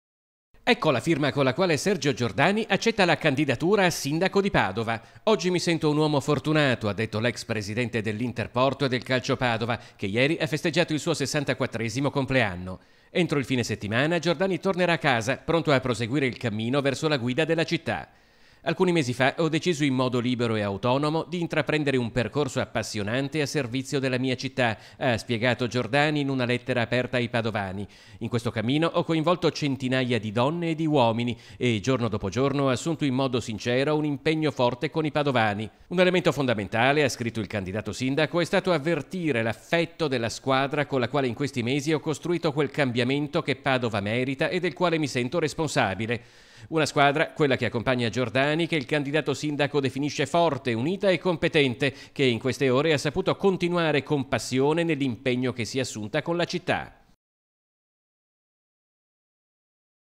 TG PADOVA (11/05/2017) - SERGIO GIORDANI FIRMA LA CANDIDATURA A SINDACO
TG PADOVA (giovedì 11 maggio 2017) - Ecco la firma con la quale Sergio Giordani accetta la candidatura a sindaco. “Oggi mi sento un uomo fortunato” ha detto l’ex presidente dell’Interporto e del Calcio Padova che ieri ha festeggiato il suo 64esimo compleanno. Entro il fine settimana tornerà a casa pronto a proseguire il cammino verso la guida della città
Alcuni mesi fa ho deciso in modo libero e autonomo di intraprendere un percorso appassionante a servizio della mia città. – ha spiegato Sergio Giordani in una lettera aperta ai padovani – In questo cammino ho coinvolto centinaia di donne e di uomini e giorno dopo giorno ho assunto, in modo sincero, un impegno forte con i padovani e oggi ne sento, più che mai, tutta la responsabilità.
Un elemento fondamentale – ha scritto il candidato sindaco – è stato avvertire così forte l’affetto della squadra con la quale, in questi mesi, ho costruito quel cambiamento che Padova merita e del quale mi sento responsabile. Una squadra definita forte, unita e competente che, anche in queste ore, ha saputo continuare con passione nell’impegno che si è assunta con la città.